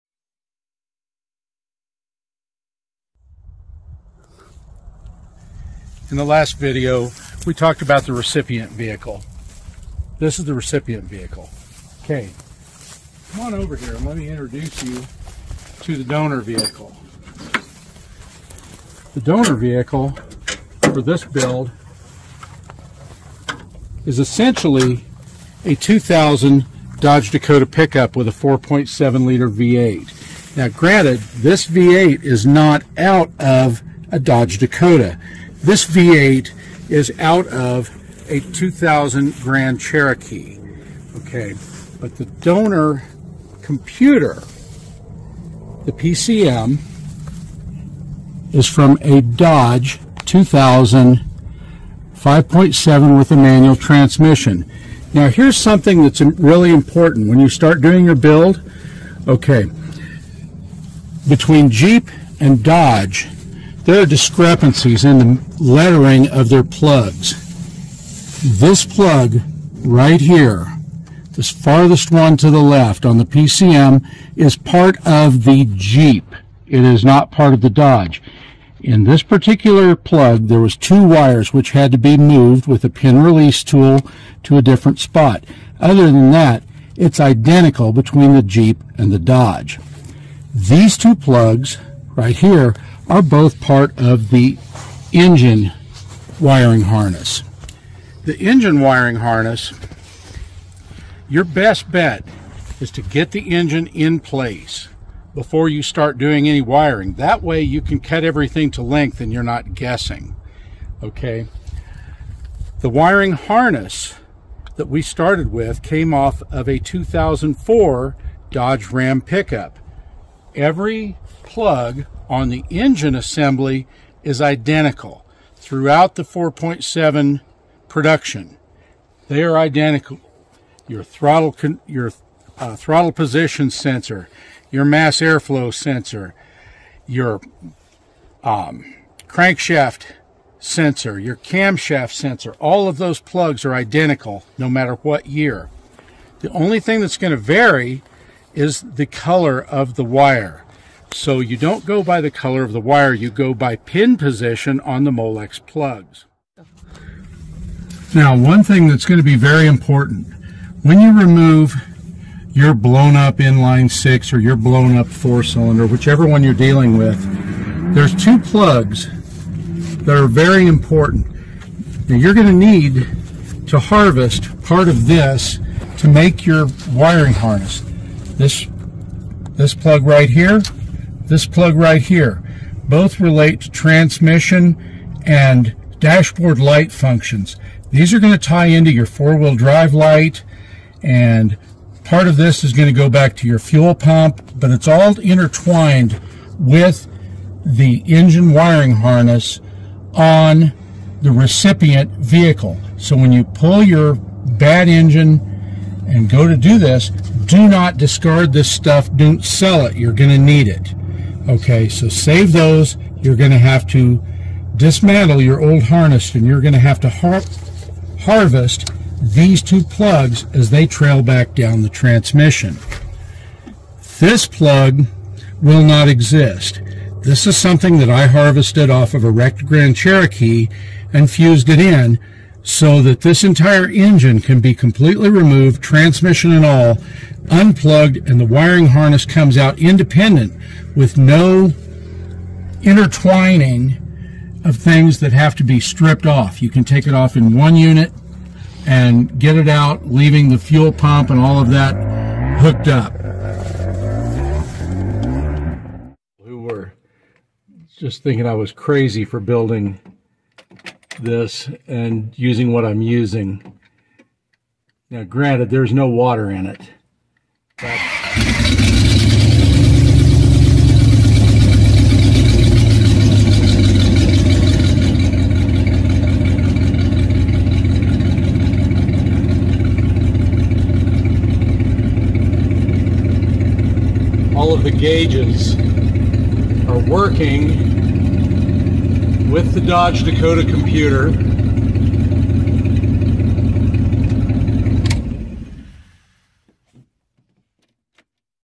4.7 Liter V8 in a TJ....WHY ???
Need to know information for transplanting a Chrysler V8 into your Jeep TJ